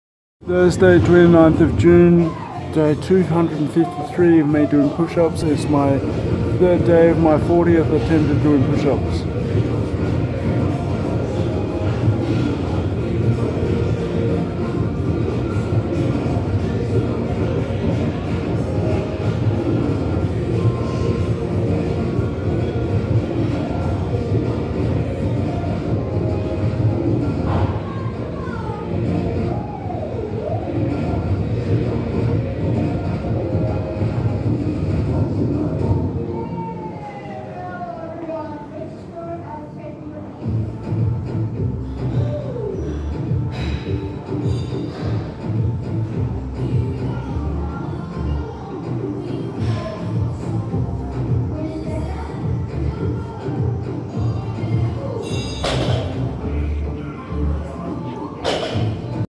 ⭕Thursday 29nd June, 2017 -day 3 of 40th attempt of doing push-ups; day 253 of doing push-ups - 30 s
⭕Thursday 29nd June, 2017 -day 3 of 40th attempt of doing push-ups; day 253 of doing push-ups - 30 standard Liam push-ups×3🤔; 40 standard Liam push-ups×1🙂
; 35 standard Liam push-ups×40☺; 43 standard Liam push-ups×1;
208🤗